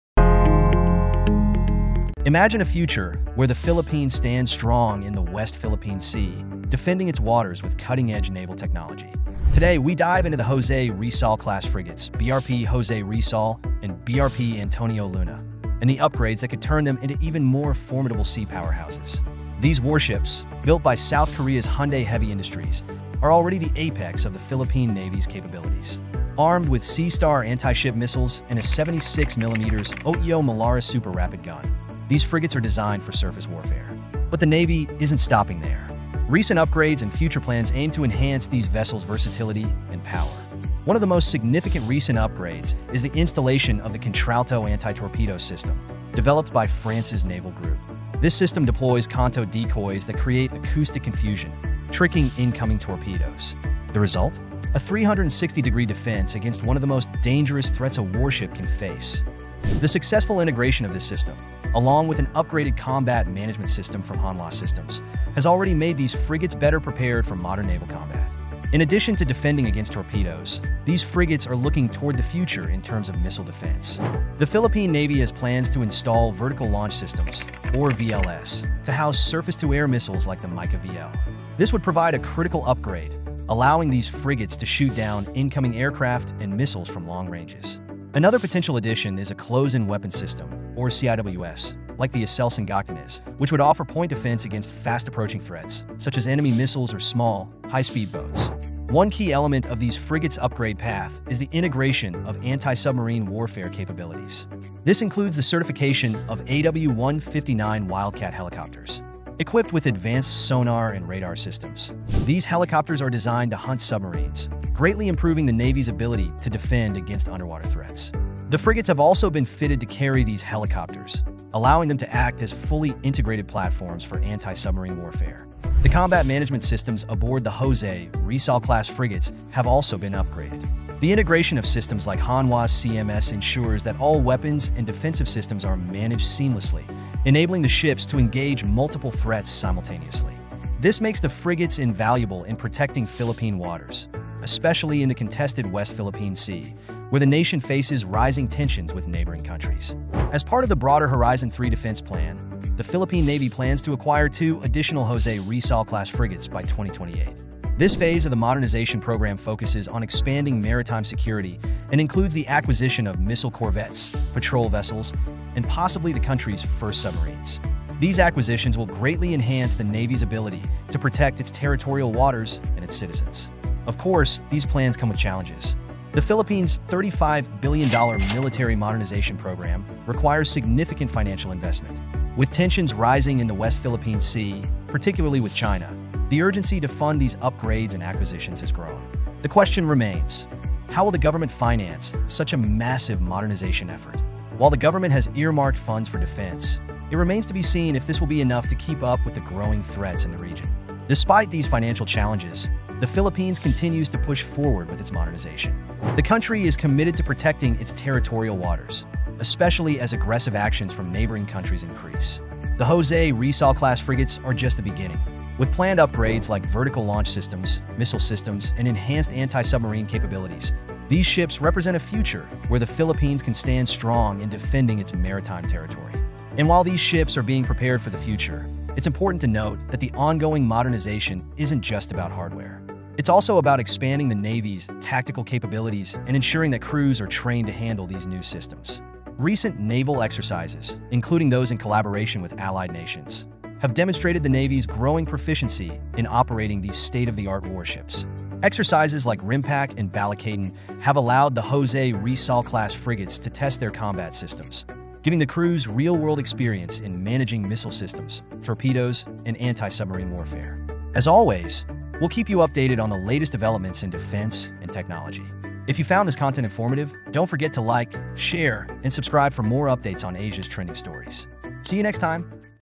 Philippine Navy's NEW GAME CHANGER! BRP Jose Rizal-Class Frigates UPGRADED
In this in-depth video, we explore the Philippine Navy’s BRP Jose Rizal-class frigates, the cutting-edge warships that are leading the modernization of the country’s naval defense. Learn how the BRP Jose Rizal (FF-150) and BRP Antonio Luna (FF-151) are equipped with advanced missile systems, including the C-Star anti-ship missiles and the newly installed Contralto anti-torpedo defense system. We also dive into future upgrades such as the Vertical Launch System (VLS) for surface-to-air missiles and the integration of AW159 Wildcat helicopters for enhanced anti-submarine warfare.

Discover how these frigates are becoming game-changers in maritime defense, as well as how the Philippine Navy is collaborating with international allies through naval exercises like RIMPAC and Balikatan to improve tactical proficiency and operational capabilities. As tensions in the West Philippine Sea rise, these advanced frigates play a crucial role in safeguarding the country’s territorial waters.

Stay updated with OHA News Asia for more content on defense, technology, and regional developments!